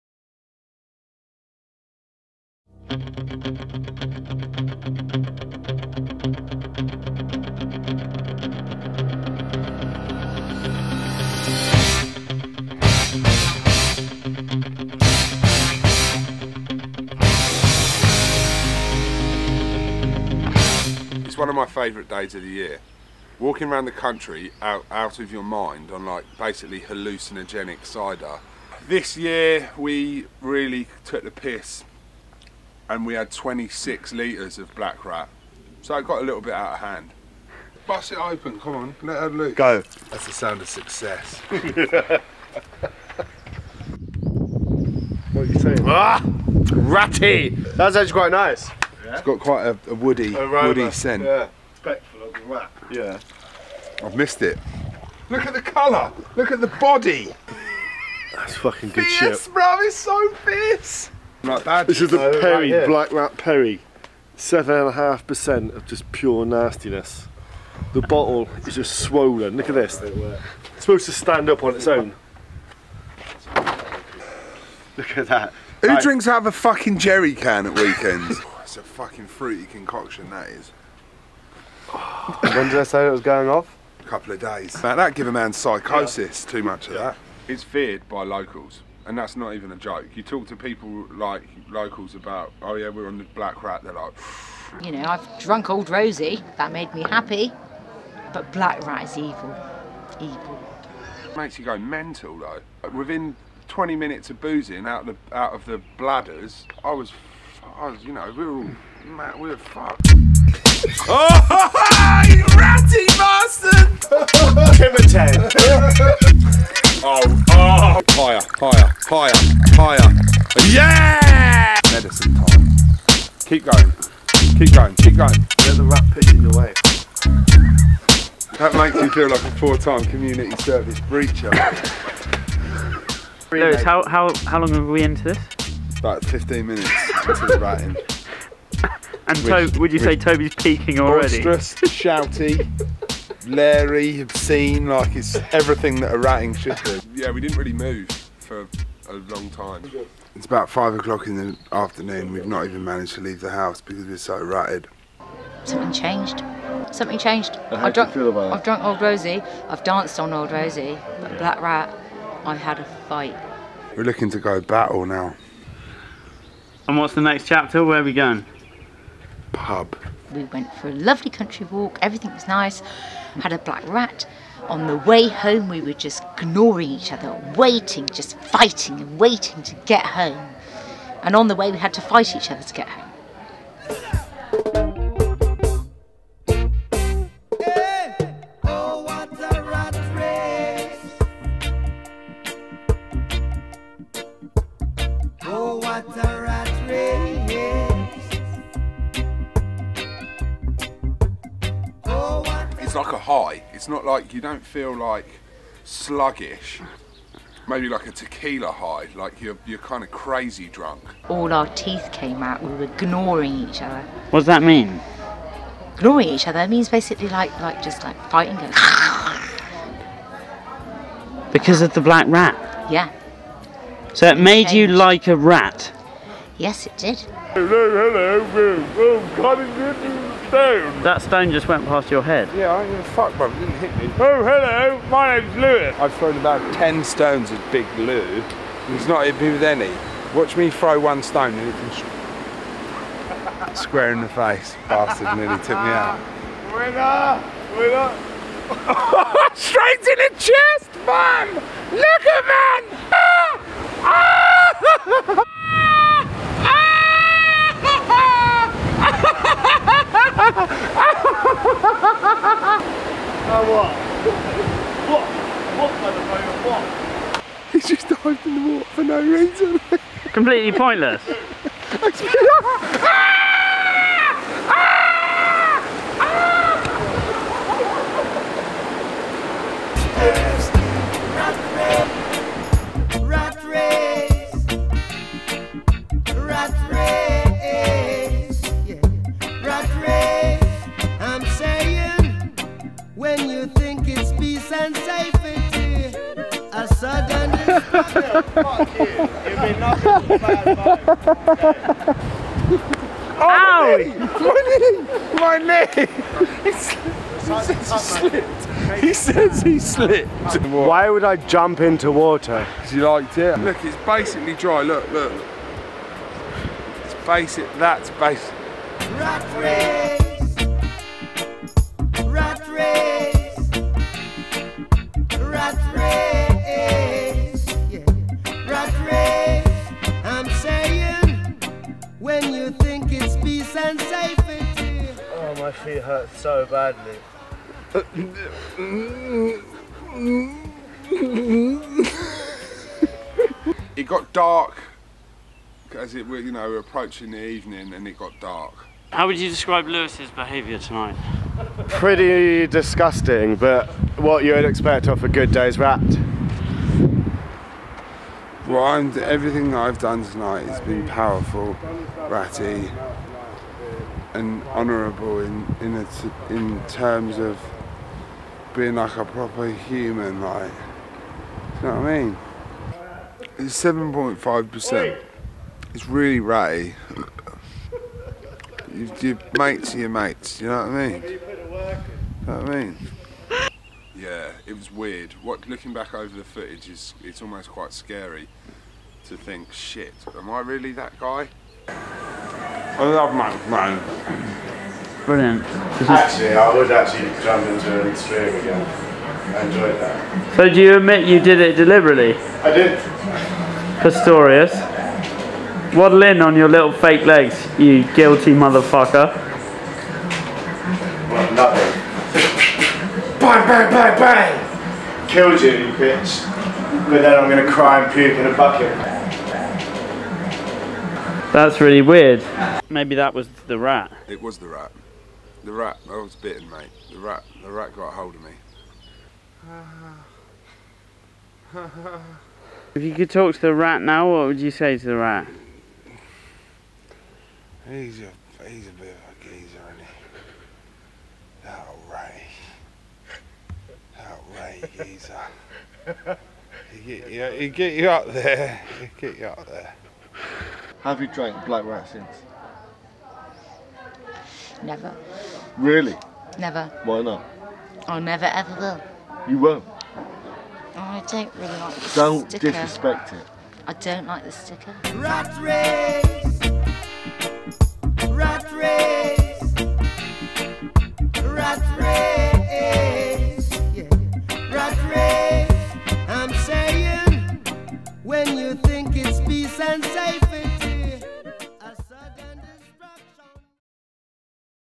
BURGER GANG Episode Five THE BLACK RAT CHALLENGE
Black Rat is a Wiltshire cider that instils fear in local drinkers. Lewis takes on the annual Black Rat Challenge as he tries to drink a silly amount of apple goodness while staying upright. Things don't go according to plan.

NB: this video contains people who are not members of Burger Gang.